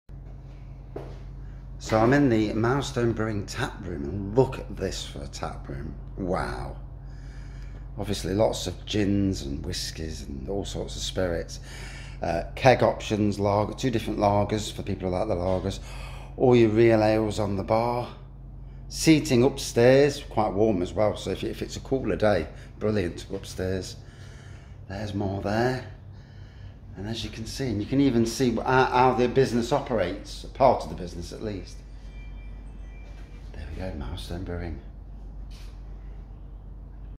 Milestone Brewery - A Look Inside the Taproom
Over to Cromwell in Newark to the Milestone Brewery and here's a look at their amazing Taproom, this is an absolute must visit place with great seating plenty of Cask and even a window to the brewery,